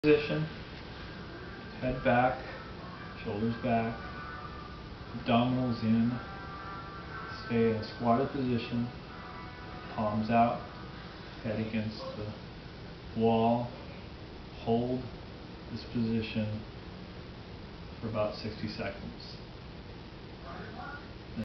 Wall Squat Video
Reduce your Back Pain Now! Perform this movement for 30 to 60 Seconds 2 to 3 times. Then stretch your quadraceps and hamstrings.